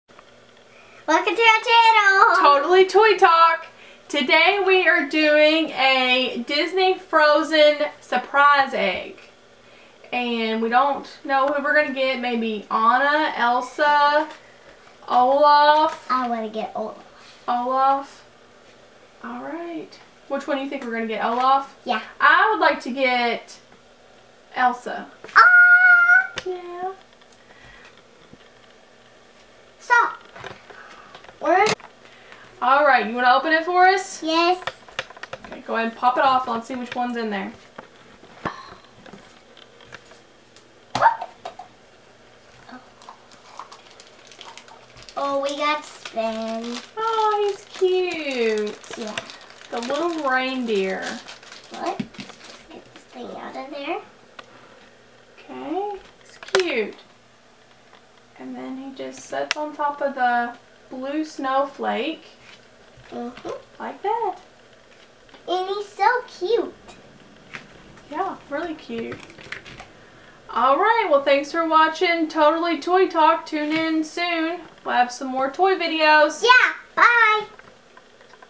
Frozen **SURPRISE EGG**
Welcome to Totally Toy Talk!

We opened a Frozen Surprise Egg. Watch the video to see which Frozen character we got!